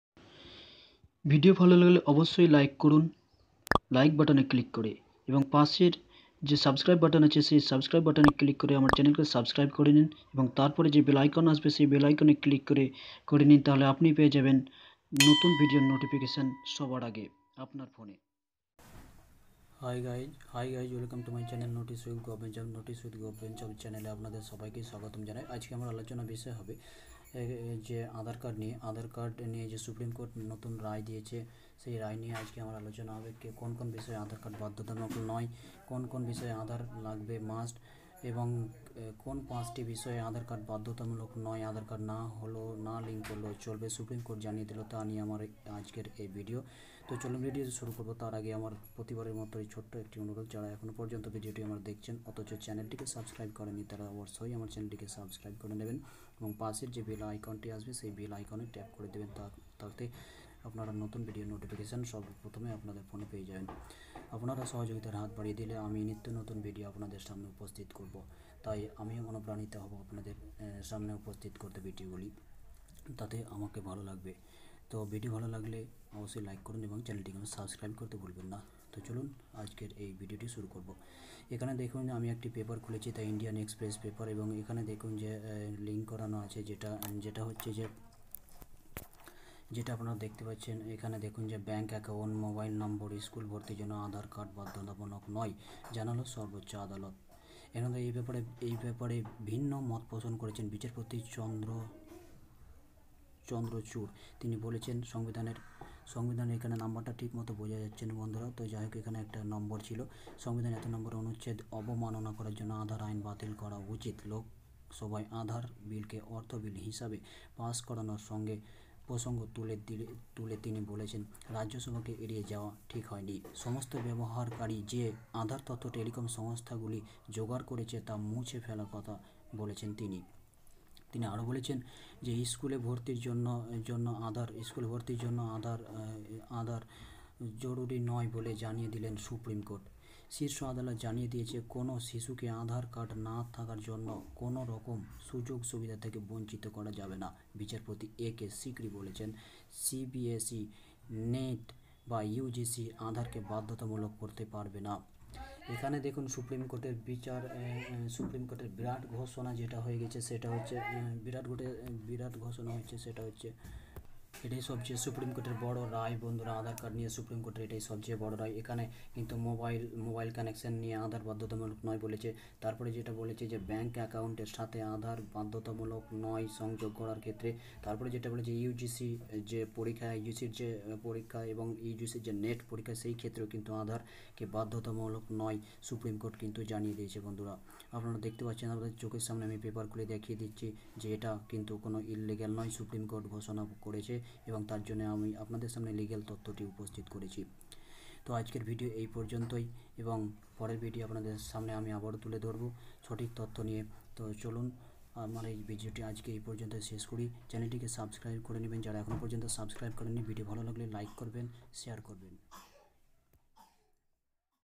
Aadhaar not mandatory/must by Supreme Court, Supreme Court aadhaar advice,aadhaar not compulsory
Aadhaar card not compulsory every stages like bank,ugc,net,mobile sim, etc. Advice by Supreme Court, must see every Indian it can help every aadhaar holder. for more video click noticewithgovtjob.